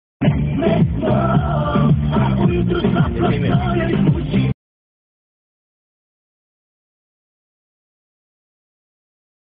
Tri izgubljene svinje lutale po brzoj cesti bez vlasnika
Izgubljenim životinjama vozači su trubili da se maknu. Svinje su bile bez nadzora, a najbliže kuće od ceste su bile udaljene kilometar